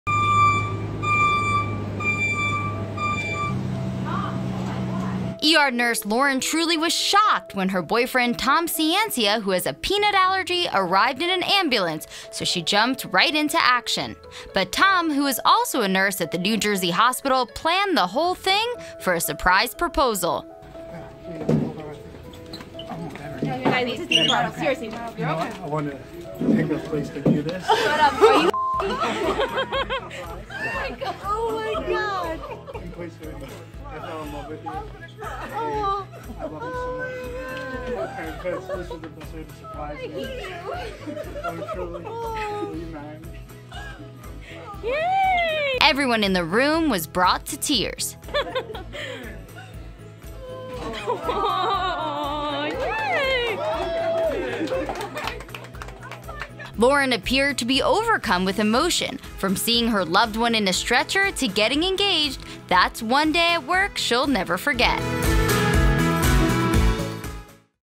Boyfriend Arrives In Ambulance With Fake Emergency For Surprise Proposal To Nurse
This is one day at work a New Jersey nurse is bound to never forget thanks to her now fiance. Nurse Lauren Trulli had the shock of a lifetime when her boyfriend Tom Ciancia, who has a peanut allergy, arrived up at the hospital in an ambulance. Lauren had no idea her boyfriend was going to surprise her with a proposal.